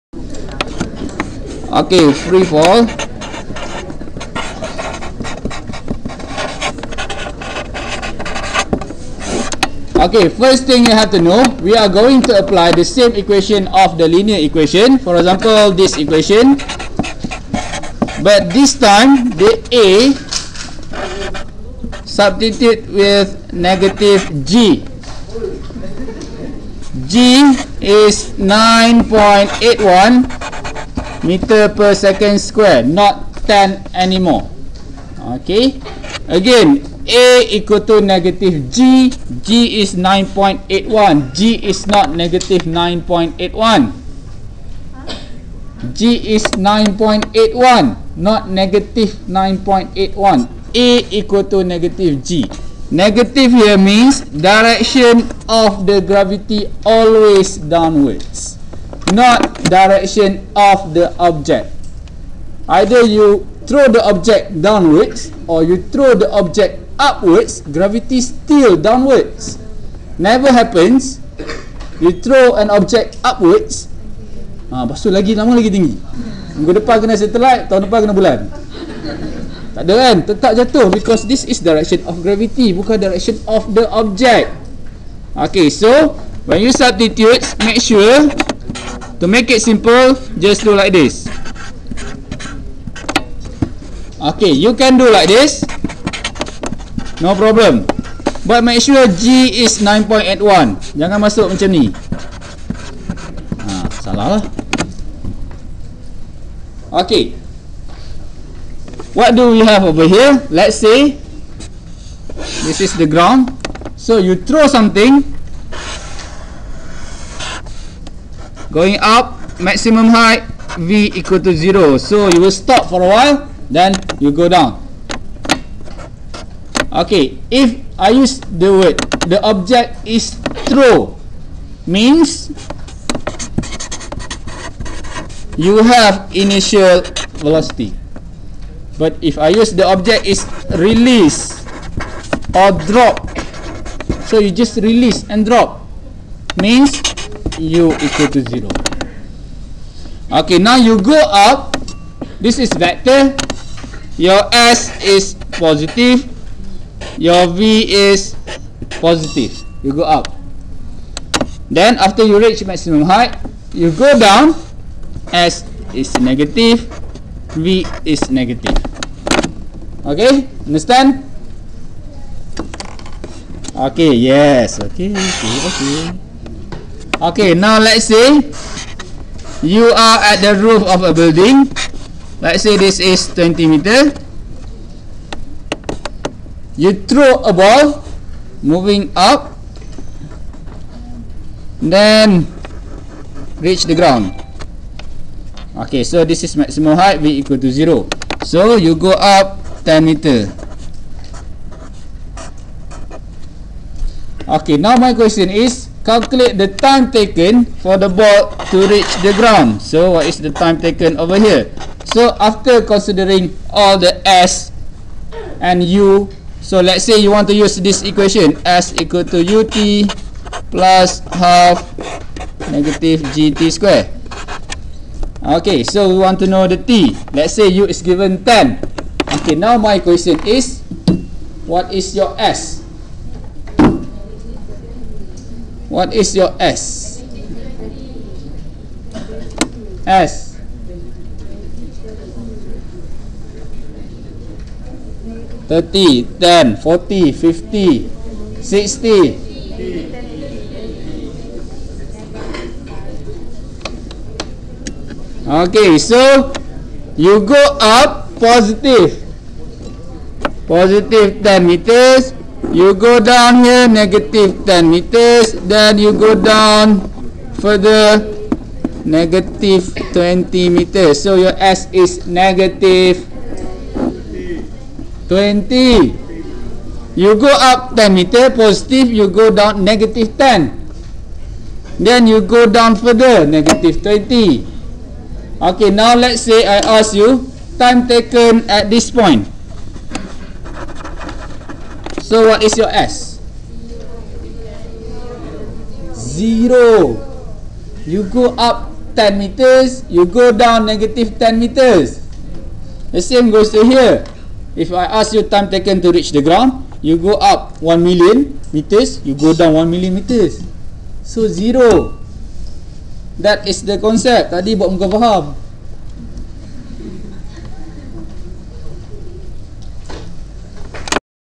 Free falling body KMS_yusfi
Explanation of free fall concept for matriculation KPM Program